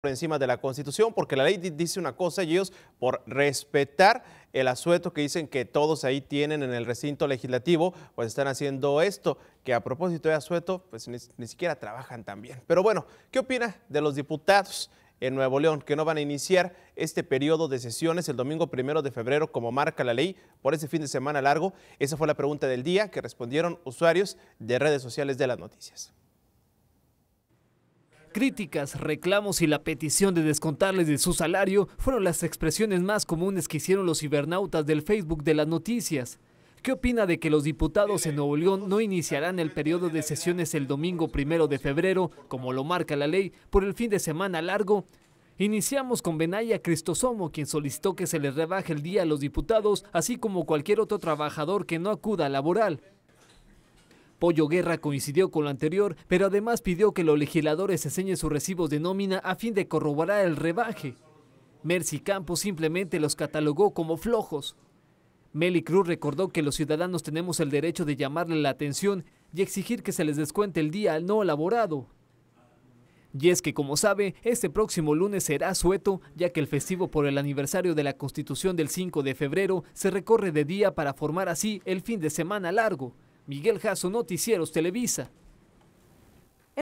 Las Noticias - Exigen descontar falta a diputados mediante redes sociales
¿Qué opina de que los diputados en NL no iniciarán el periodo de sesiones el domingo 1 de febrero, como marca la ley, por el fin de semana largo? se les cuestiono a los seguidores de Las Noticias.